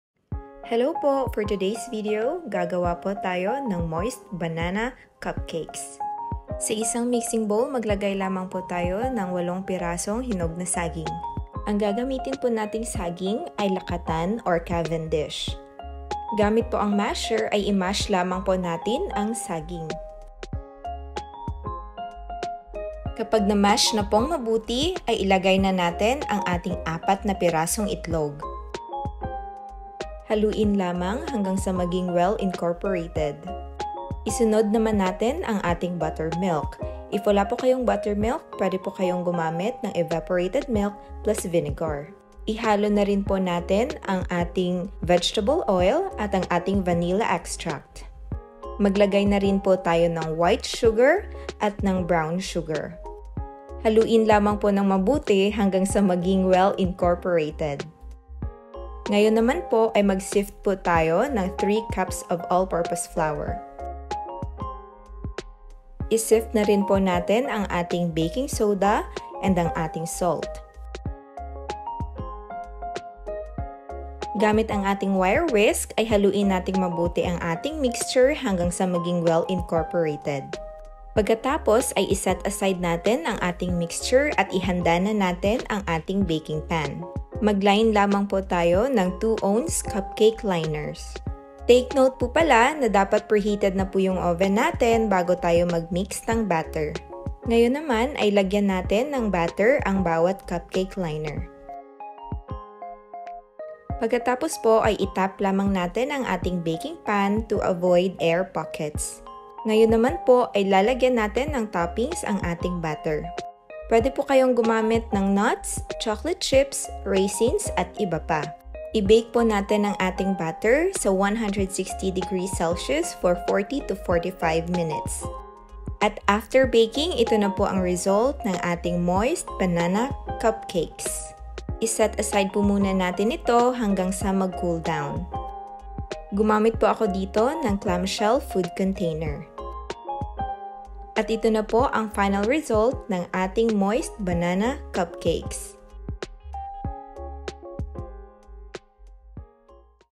EASY & DELICIOUS Moist Banana Cupcake
Moist Banana Cupcakes

Ingredients:

▫️8 ripe bananas (preferred yung hinog na hinog na talaga, yung naglalaglagan na… mas matamis yun ☺️)
▫️1 cup buttermilk (alternative: 1 cup evaporated milk + 1tbsp vinegar)

Toppings:
I used chocolate chips because that’s the only available in my kitchen. ☺️ But you can also use the following:
▫️Nuts
▫️Raisins
▫️Streussels
▫️ Sliced bananas

Bake @ 160C for 40-45 minutes or until golden brown.

NOTE: Preheat the oven before making the batter to ensure that the oven is preheated well.